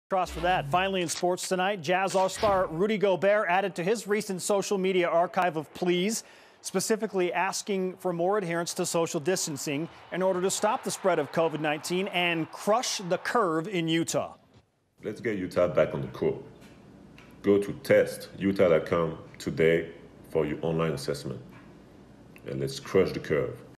Rudy Gobert Appears In New TestUtahChallenge PSA for Coronavirus
Rudy Gobert sent out a tweet on Thursday asking Utahns to go to testutah.com to take part in an online assessment to better understand how COVID-19 has impacted the state of Utah, and whether they should seek testing.